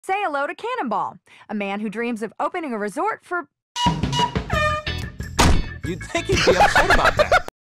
[Vinesauce] Vinny - Say Hello To Cannon Ball! (FAN CLIP)
Vinesauce Vinny Laughs at the tragic death of a Man who just wanted to open a resort for. Hilarity Ensues!